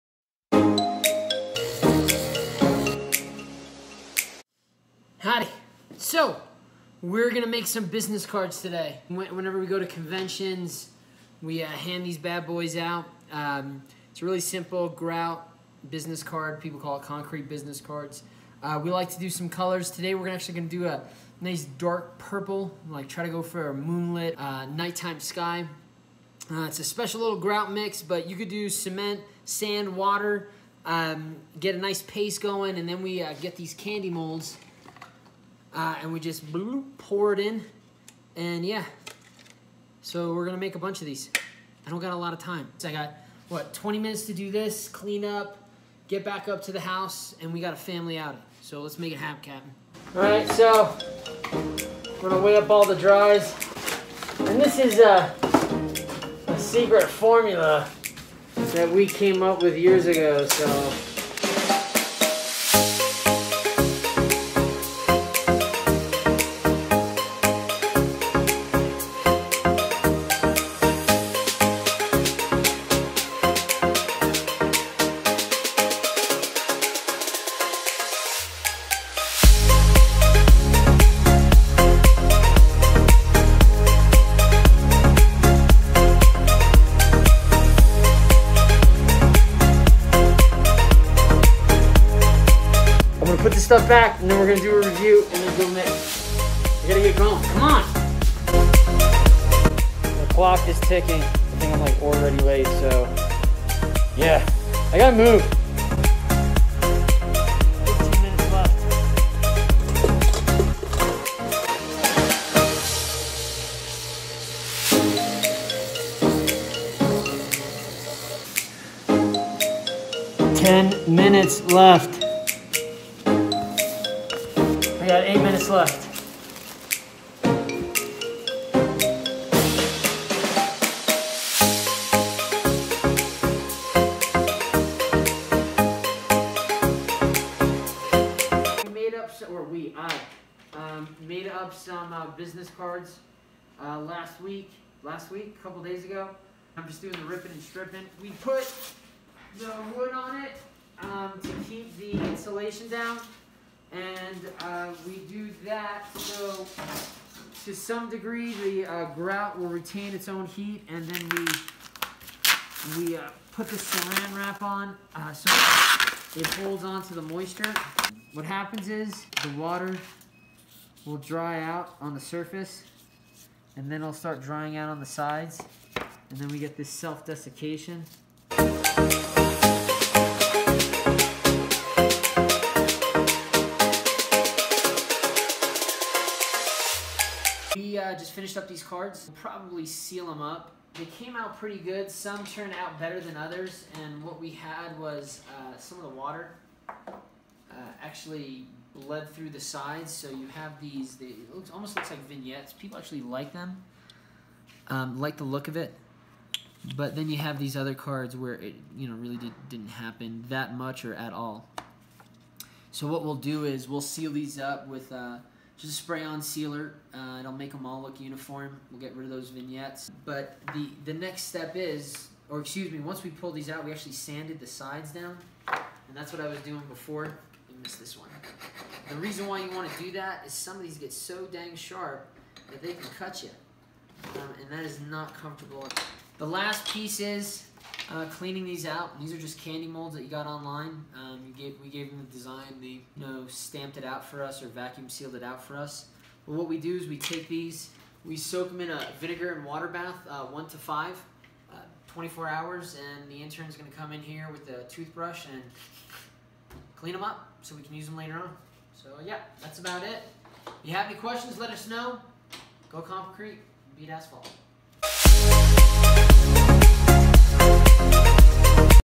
Concrete Business Cards! - Vlog #25
Jon is in the lab making some concrete business cards to hand out at conventions!